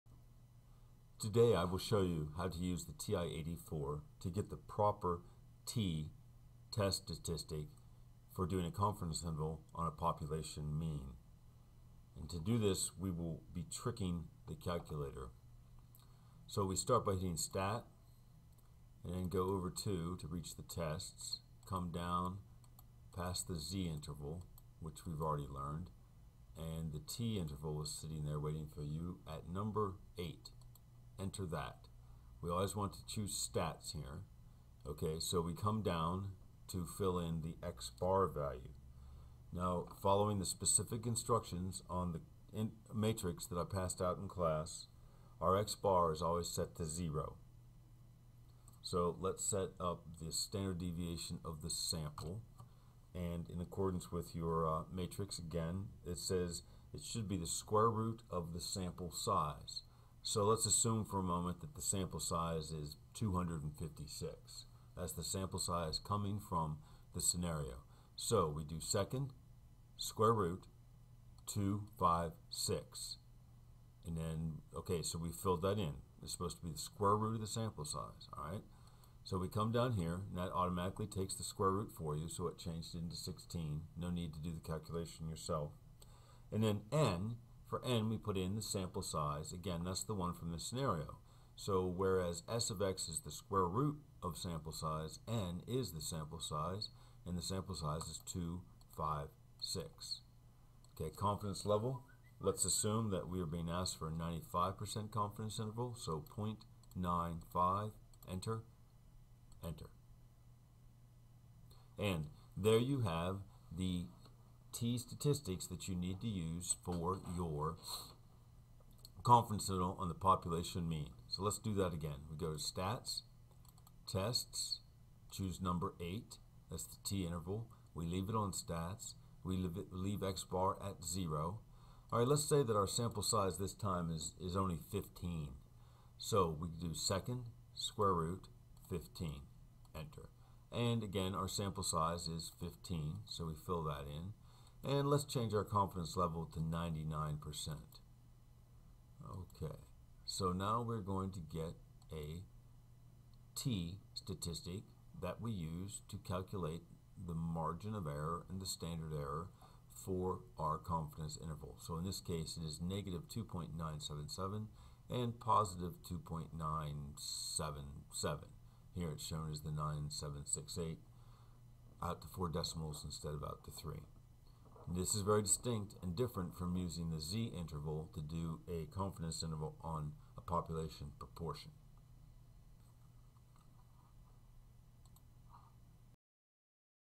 Finding t test statistic for confidence interval on TI 84 or 83 (critical value)
Finding t test statistic (critical value) for confidence interval on TI 84.  For population means only